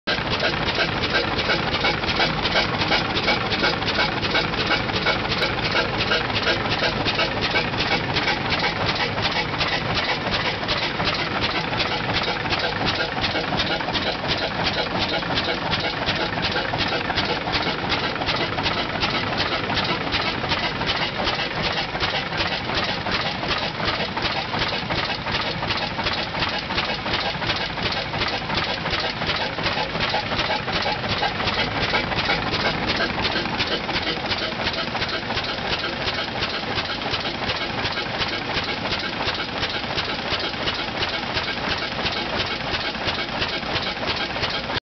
1912 Rumely 15-30 Model F powering 36 inch thresher.
1912 Rumely 15-30 Model F powering the 36 inch Nichols and Shepherd thresher at the South Peace Centennial Museum on July 15, 2012.